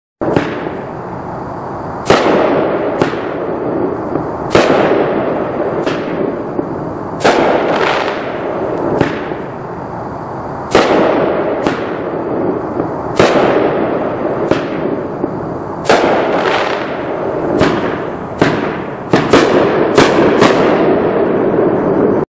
Raging Hormones
Junior Power Series-300+ Gram
Red tail to red star and white strobe; green tail to green, purple star; silver tail to blue and gold strobe; blue tail to colorized star and time-rain; the last three shots of blue peony and silver strobe pistil.